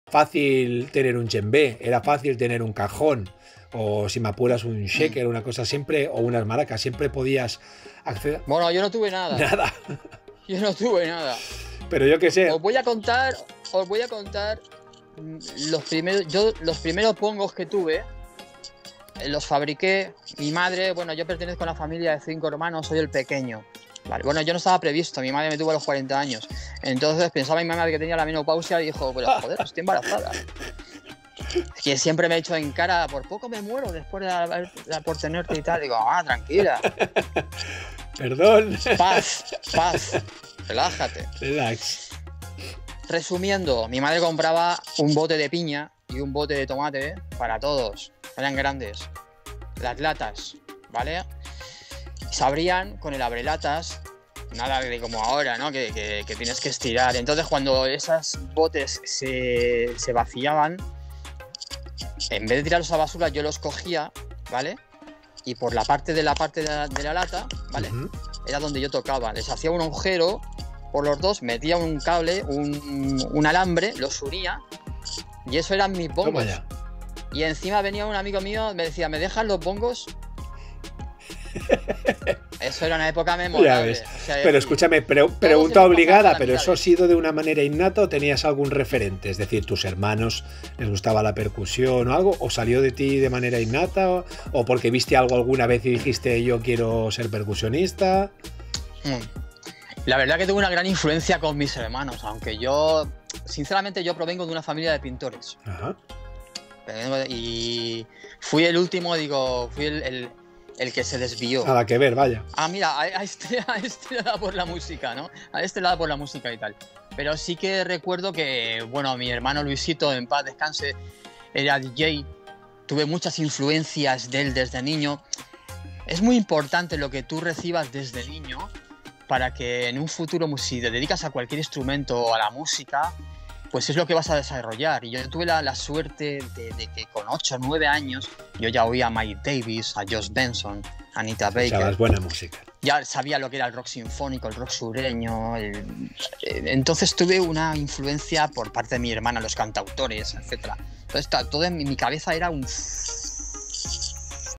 Pakito Baeza: "Empecé a tocar 🪘con LATAS DE PIÑA Y DE TOMATE"
Entre las múltiples anécdotas suyas que descubrimos en esta entrevista, nos quedamos impresionados al descubrir que sus primeros bongos fueron creados de forma artesanal, con botes de piña y de tomate.

Y como siempre, muchas gracias por seguirme en las diferentes redes y estar al día de todas la novedades!